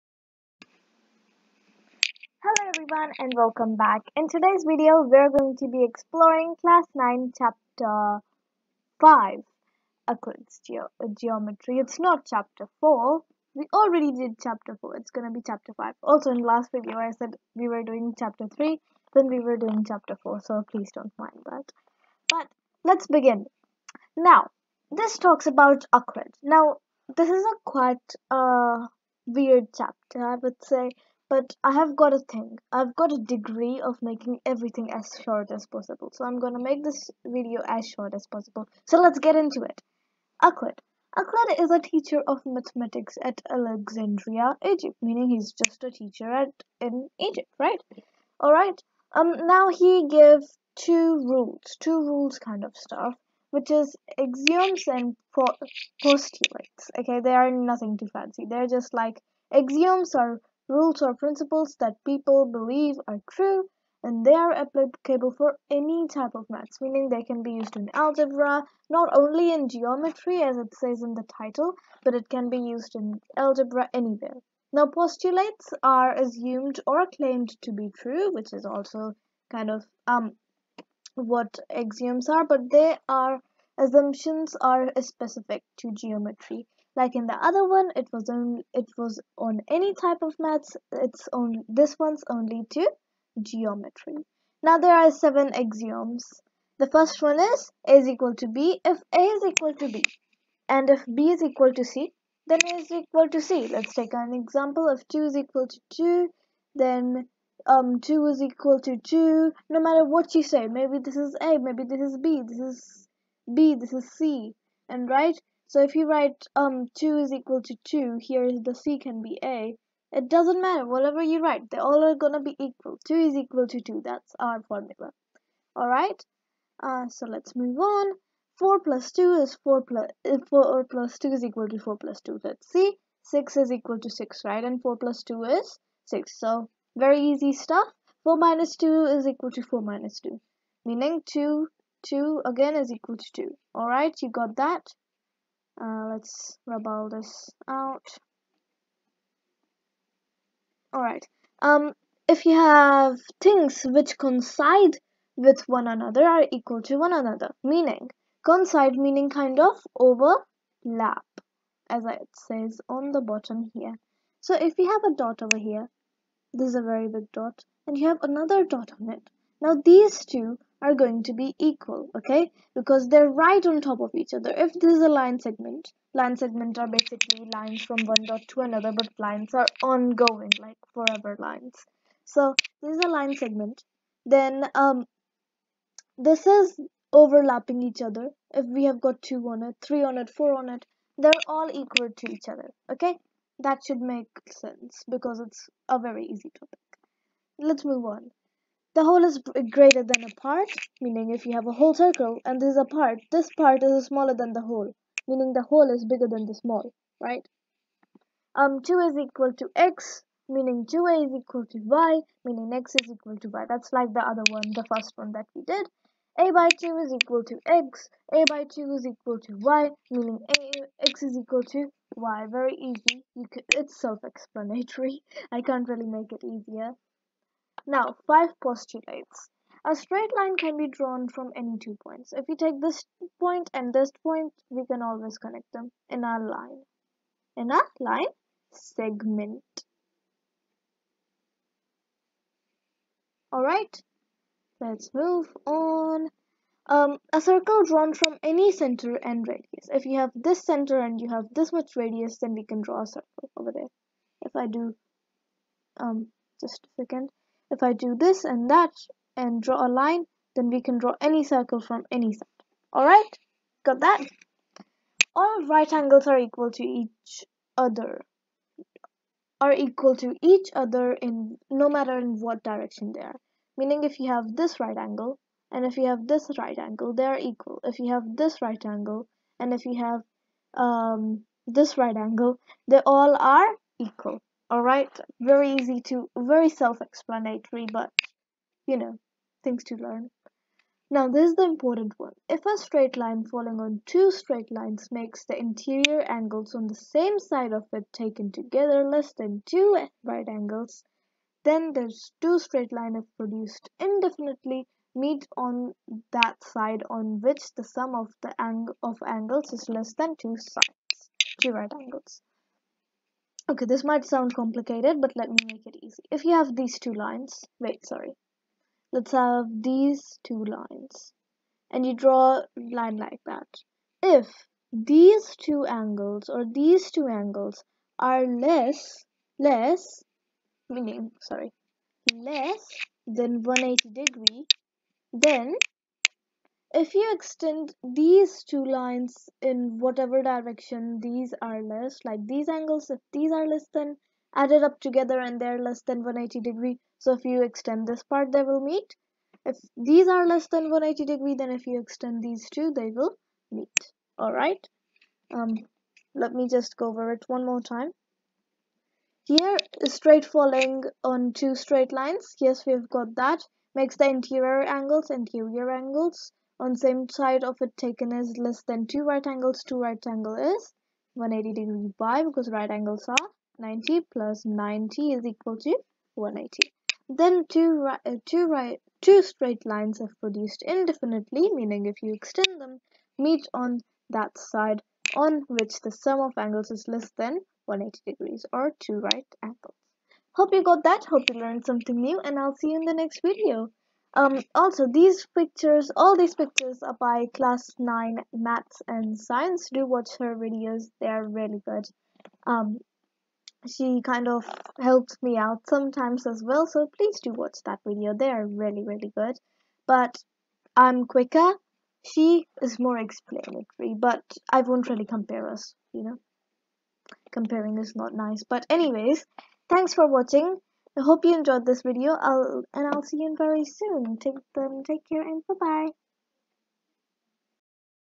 Class 9 Maths Chapter 5: Euclid's Geometry
Hello!

Euclid: A teacher of Mathematics at Alexandria egypt

Axioms: Rules or principles that people believe are true. These are applicable for any type of maths.

Postulates: They are assumed or claimed to be true. Assumptions specific to geometry.

7 axioms:

 • A=B, B=C, A=C
 • 4 + 2 = 4 + 2
 • 4 - 2 = 4 - 2
Coincide - Overlap

 • The whole is greater than part.
 • 2a = x, 2a = y, x = y
 • a/2 = x, a/2 = y, x = y

5 postulates:
 • A straight can drawn from any 2 points.
 • A circle drawn from any center and radius.
 • All right angles are equal to each direction.